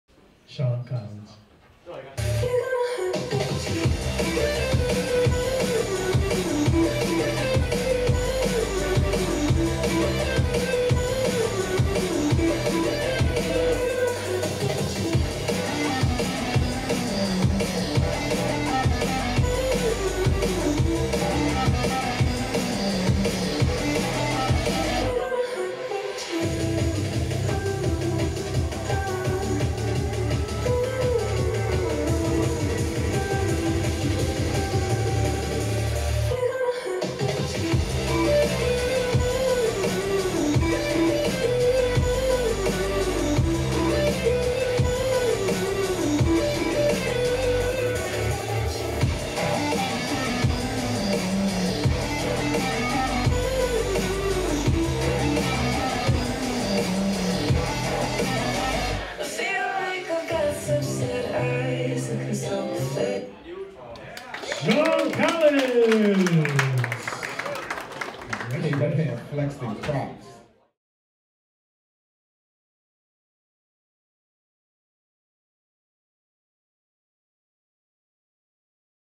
Sean Collins - 1A Prelim - 26th Place - NY States 2023 - Presented by YoYo Contest Central
NYSYC The 2023 New York State Yo-Yo Competition a.k.a. the “Coney Island Yo-Yo Fling” was held on Saturday, November 18th at Coney Island's famous Sideshows by the Seashore at 1208 Surf Ave in Coney Island, NY.